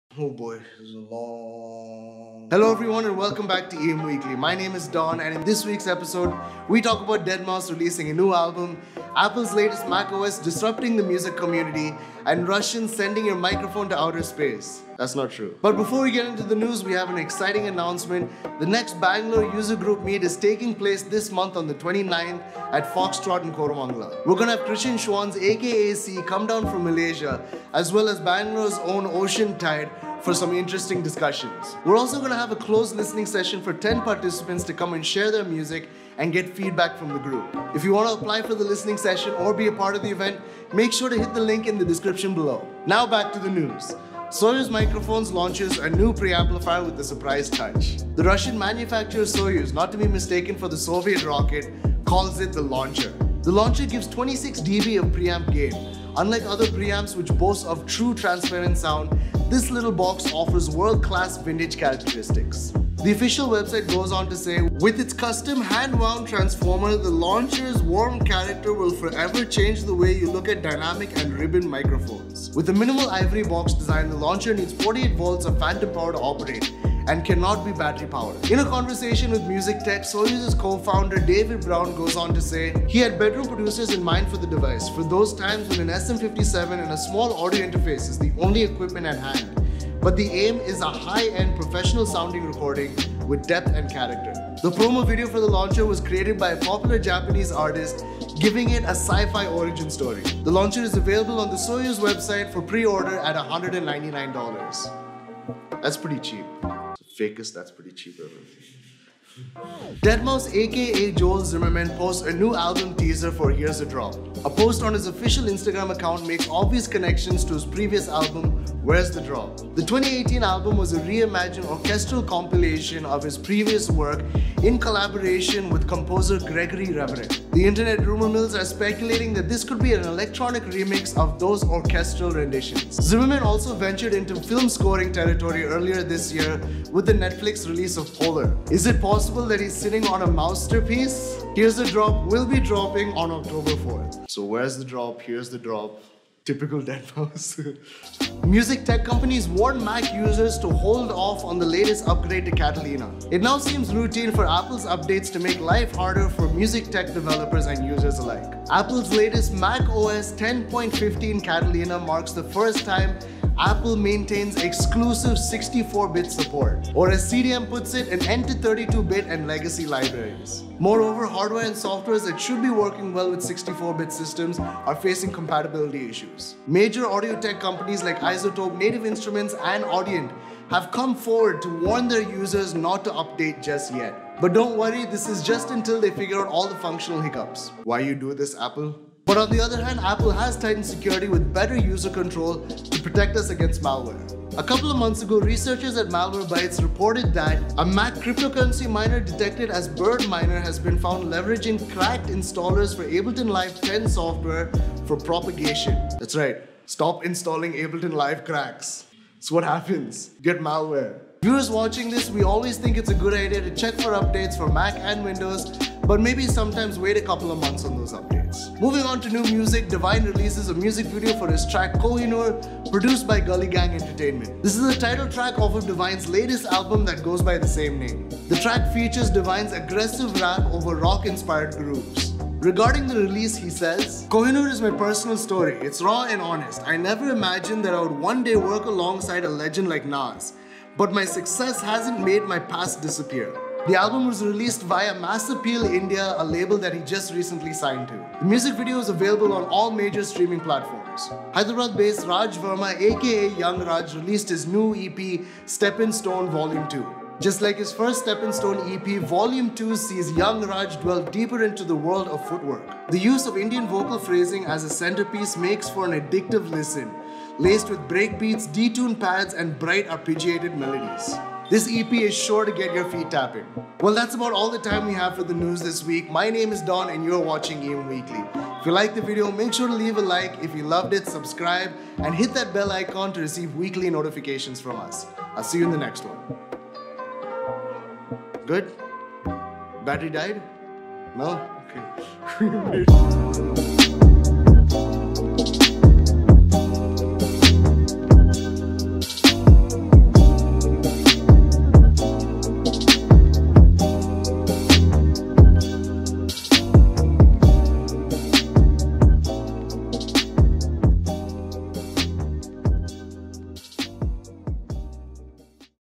New Deadmaus Album, a budget Mic Preamp and Catalina woes - EM Weekly - Ep. 17
Ableton User group Sept 2019

Main Stories -